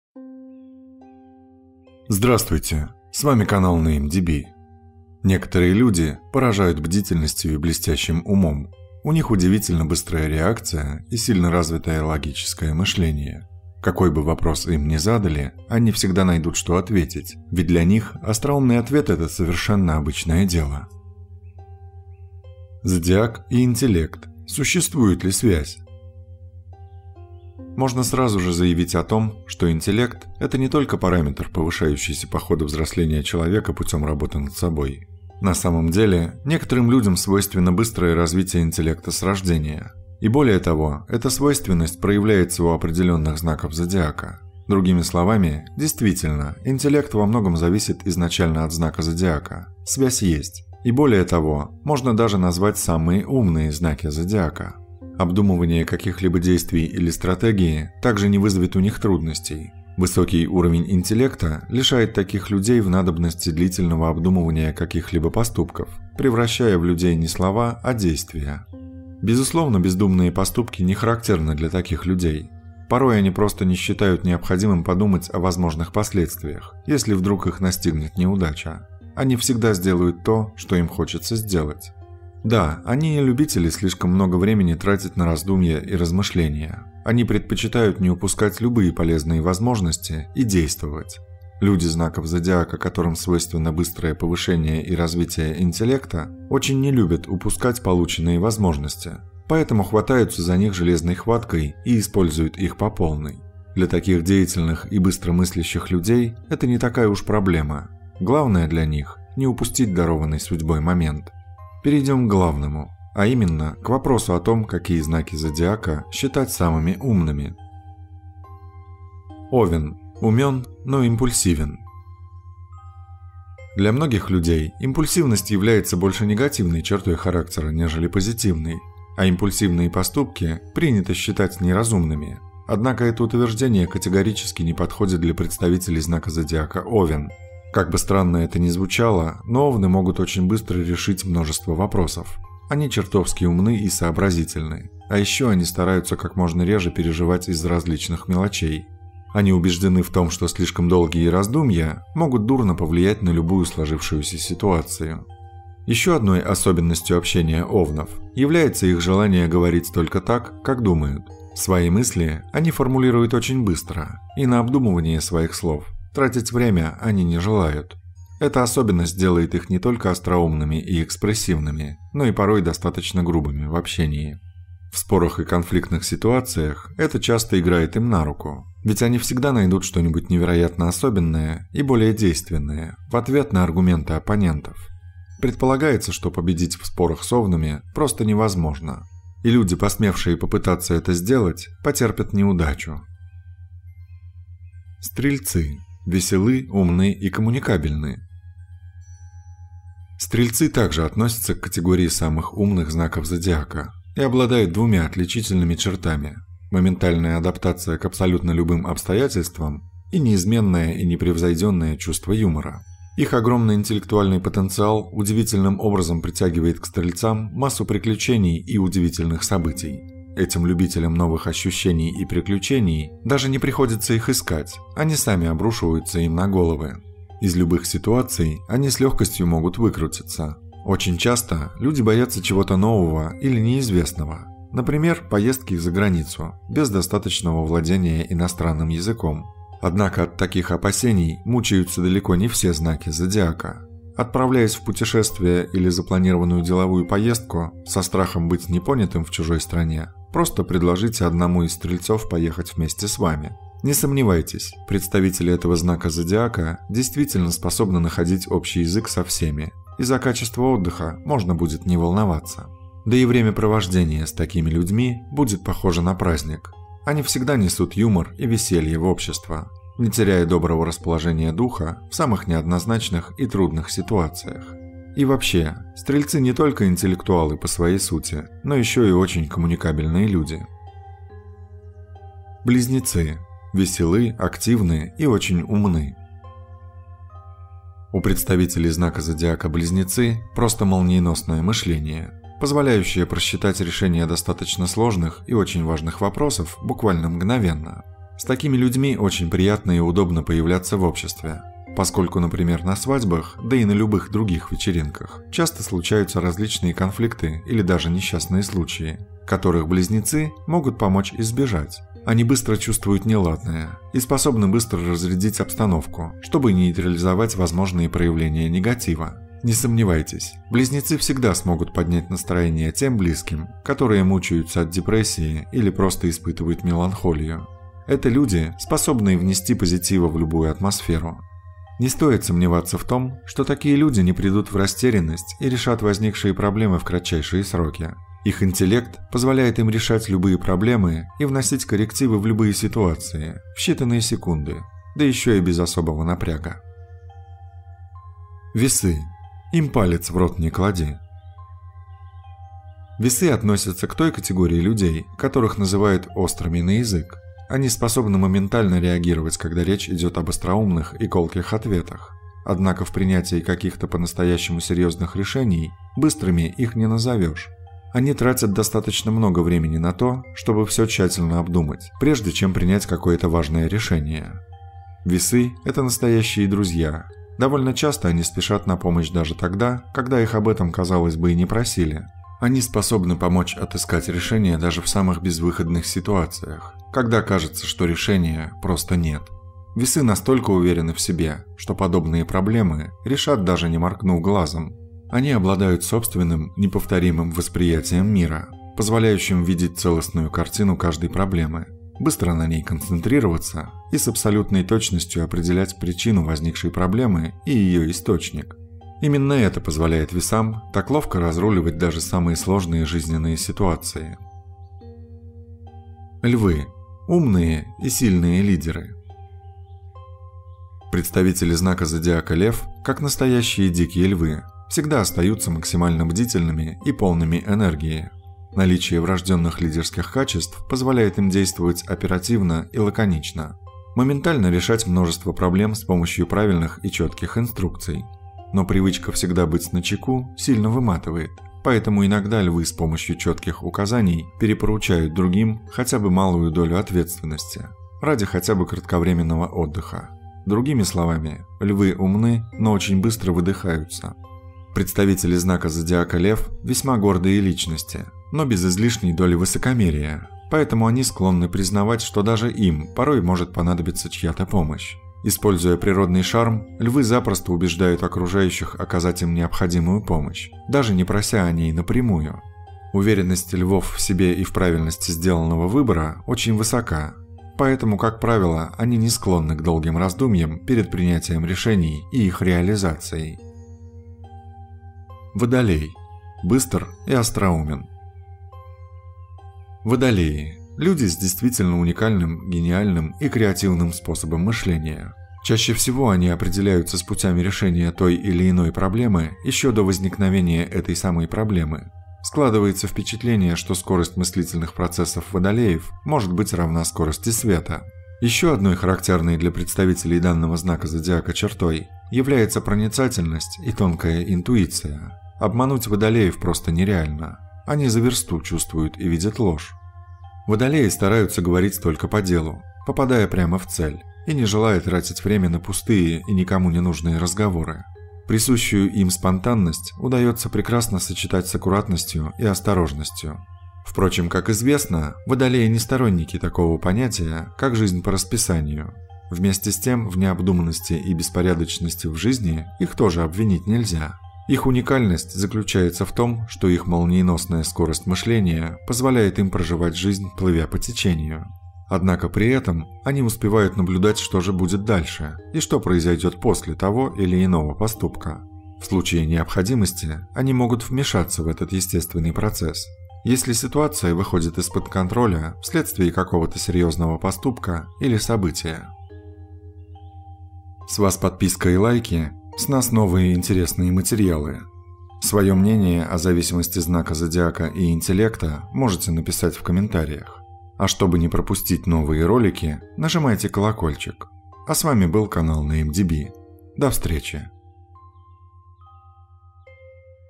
Самые умные знаки Зодиака
Некоторые люди поражают бдительностью и блестящим умом. У них удивительно быстрая реакция и сильно развитое логическое мышление. Какой бы вопрос им не задали, они всегда найдут, что ответить, ведь для них остроумный ответ — это совершенно обычное дело.